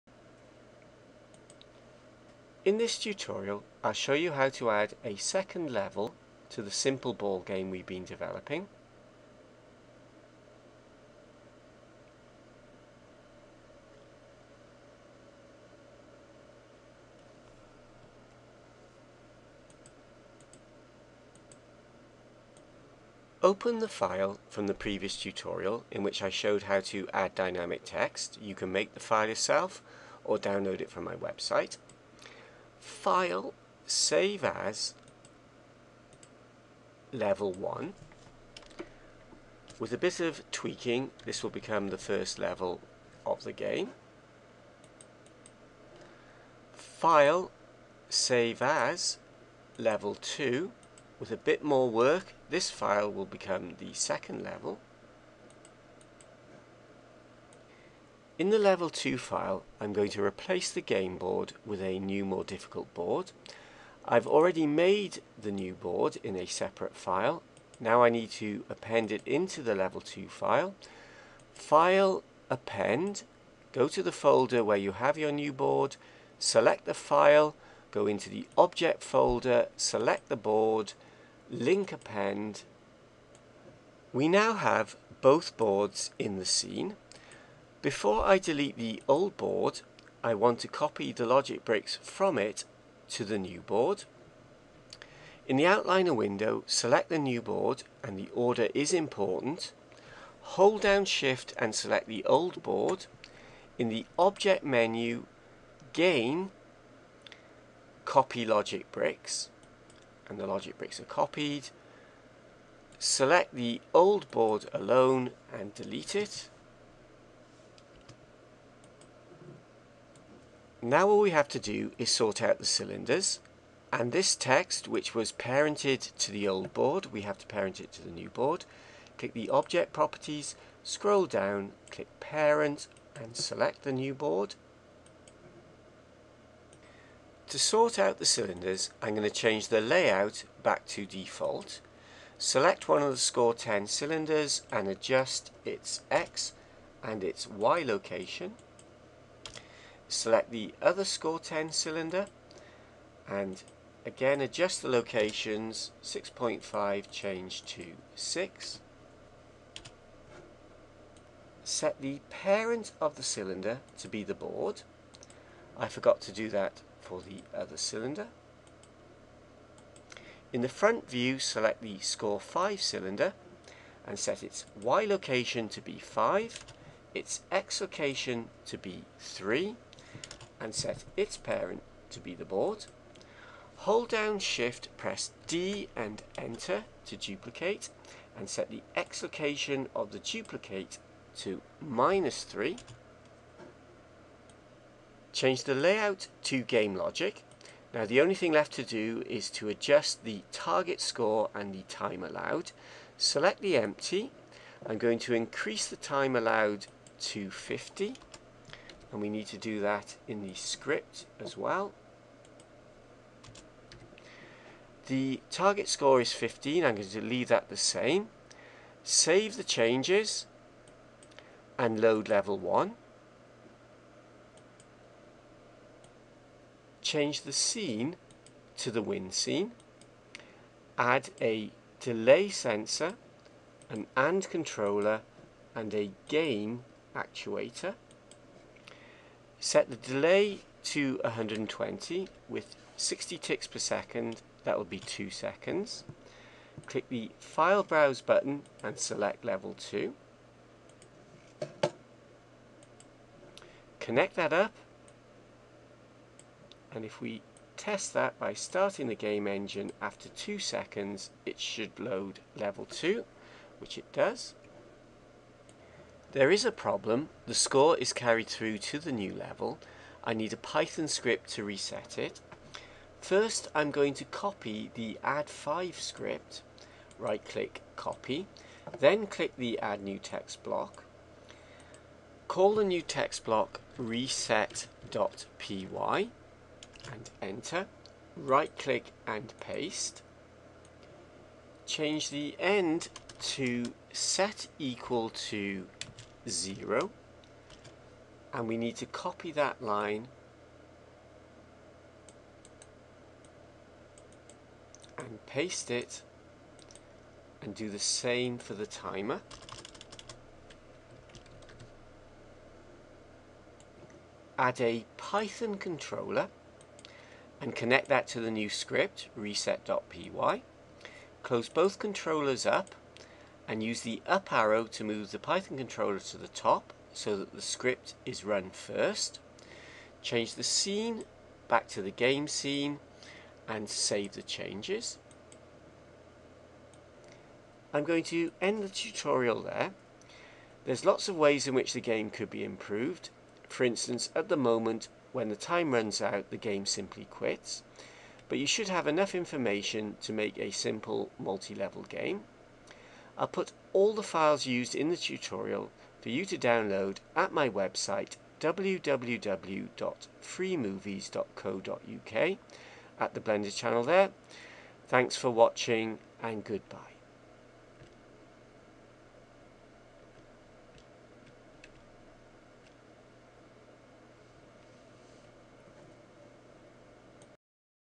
Blender Tutorial Developing a Simple Ball Game - Creating and Loading a Second Level
This is another tutorial in the simple ball game series. This tutorial shows how to append a 'harder' board into the game to create a second level.  The new level is automatically loaded when the first level is successfully completed.

The tutorial shows how to copy logic bricks from one object another i.e. from the original game board to the new board.

A Python script is needed to reset the score and timer for the new level.